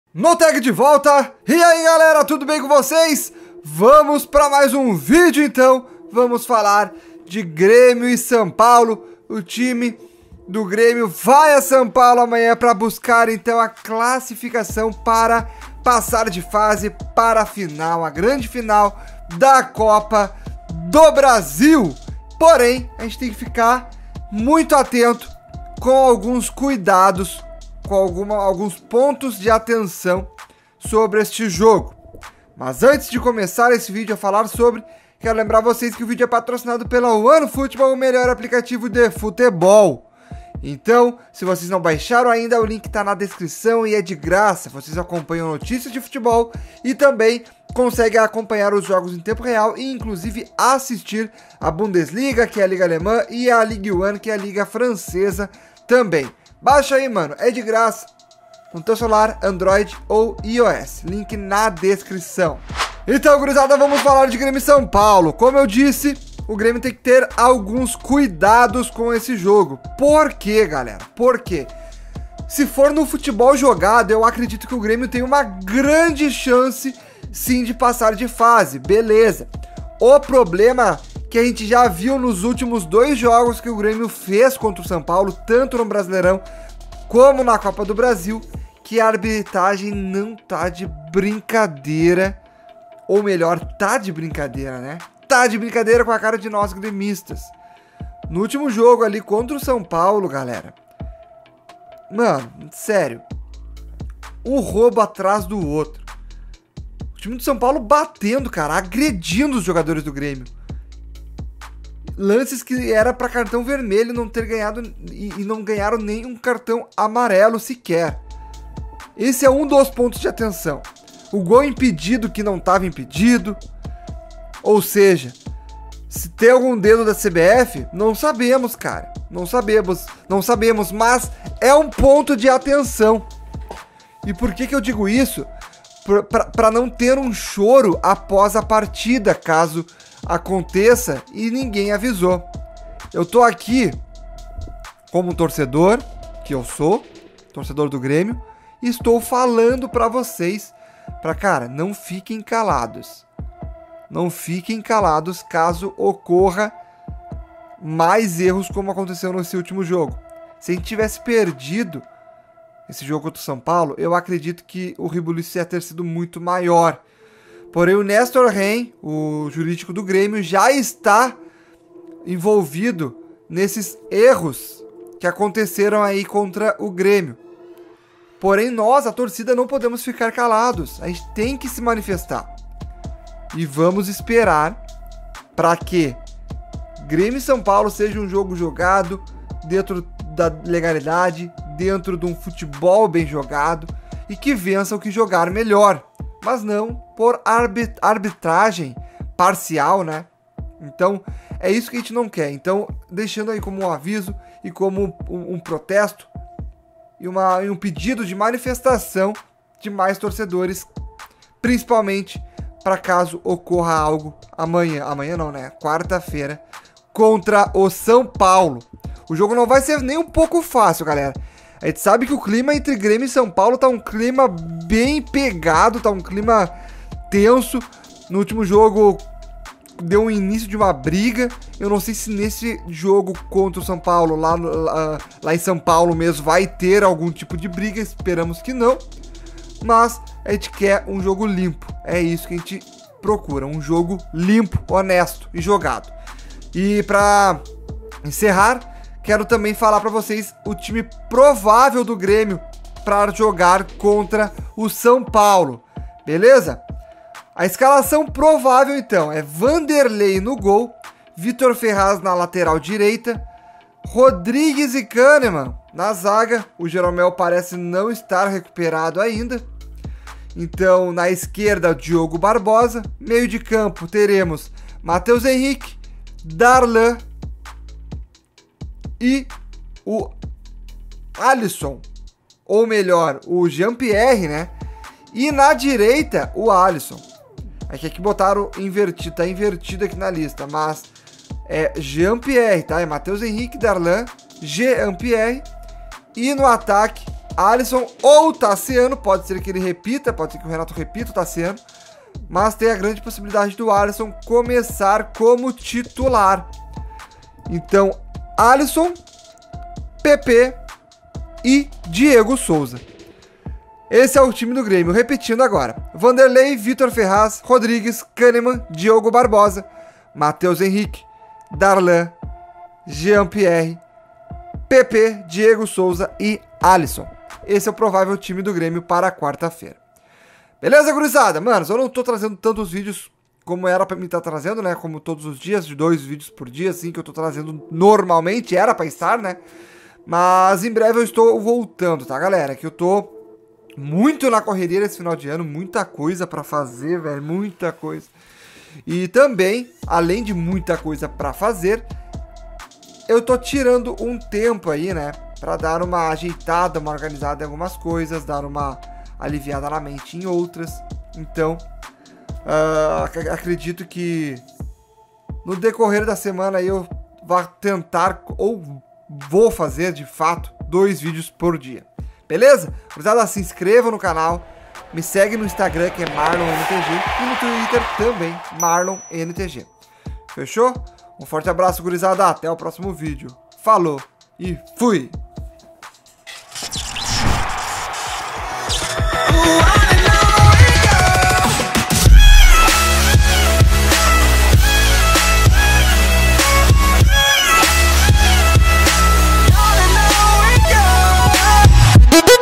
CUIDADO GRÊMIO, COM O SÃO PAULO! ATENÇÃO REDOBRADA PARA O JOGO DE QUARTA! E PROVÁVEL ESCALAÇÃO!
Atenção ao Grêmio, ao enfrentar o São Paulo, na copa do Brasil!